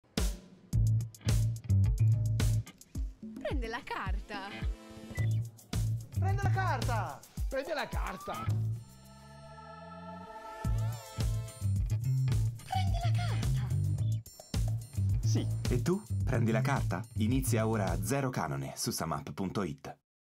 SumUp - E tu, prendi la carta?
Quando si tratta di pagare, stupisci i tuoi clienti!
Offrigli un’esperienza di pagamento incredibile, dalla colazione al bar all’acquisto del loro vinile preferito. E la capra al tuo corso di yoga non è l’unica cosa che passerà inosservata.